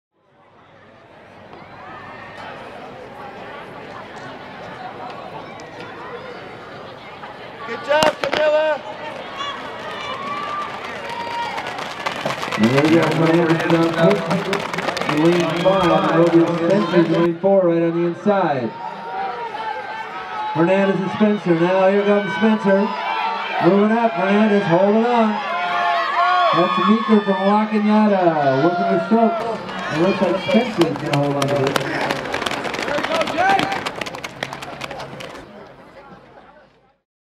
2014 RHL Prelims - 200 Meters (Varsity Men) (Heat 1)
This is heat 1 of 2 of the Varsity Men's 200 Meters qualifying race for the Rio Hondo League held at South Pasadena High School on May 6, 2014.